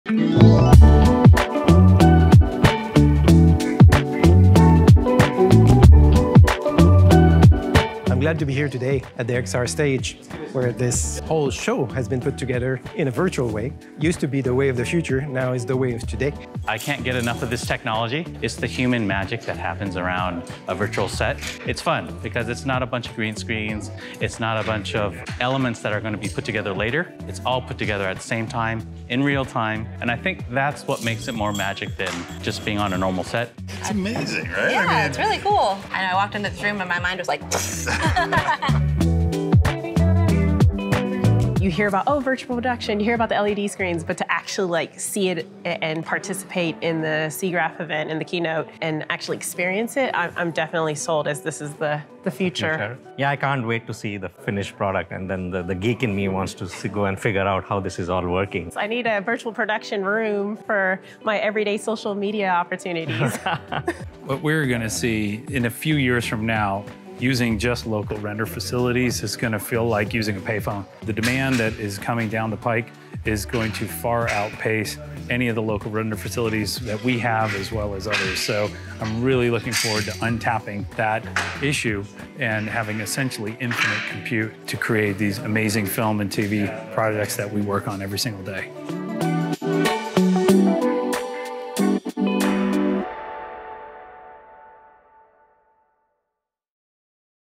Behind the scenes teaser: AWS at SIGGRAPH 2021 keynote presentation | Amazon Web Services
A short teaser for the upcoming "behind the scenes" video walks through how the SIGGRAPH 2021 keynote presentation "Through the looking glass: the next reality for content production" was created using real-time techniques on a virtual production stage with LED volume walls.

Watch all of the AWS content for SIGGRAPH 2021 on-demand